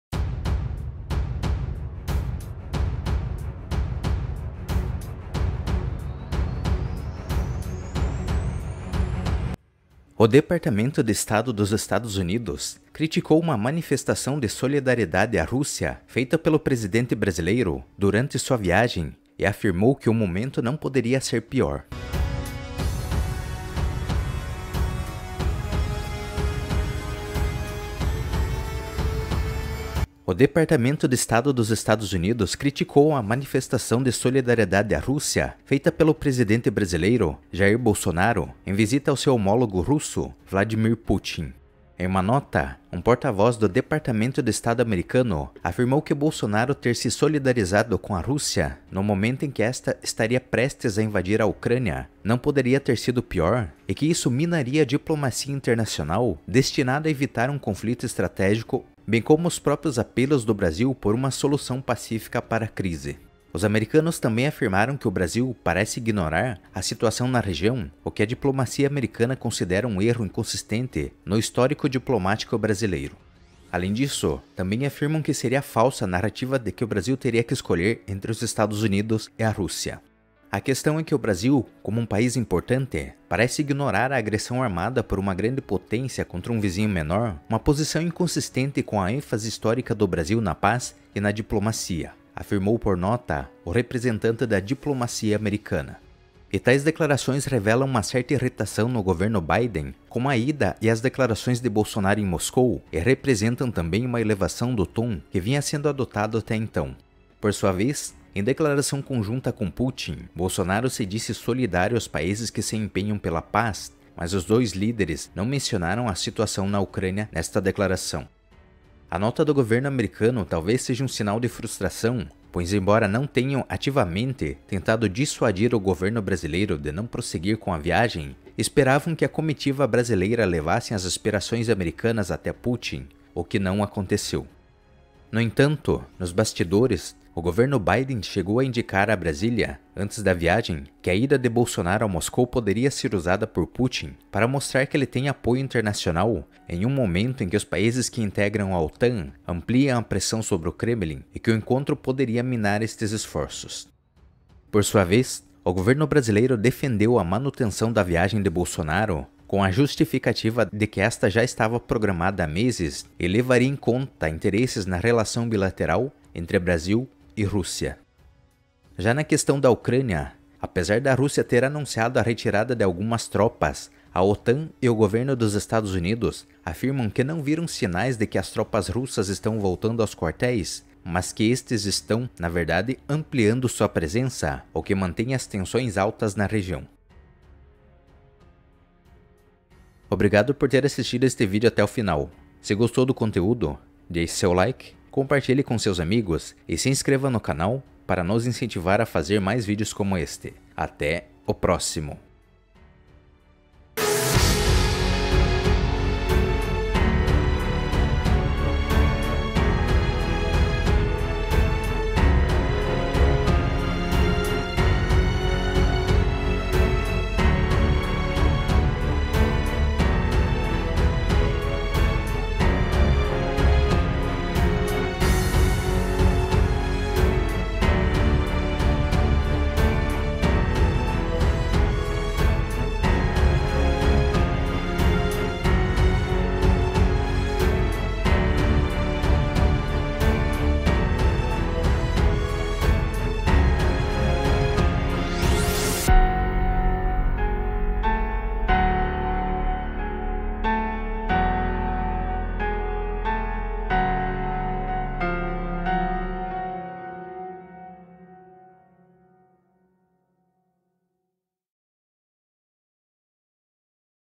Governo Biden critica fala de Bolsonaro na Rússia
Em nota, Departamento de Estado dos Estados Unidos criticaram fala de Bolsonaro na Rússia e a posição do Brasil na crise sobre a Ucrânia.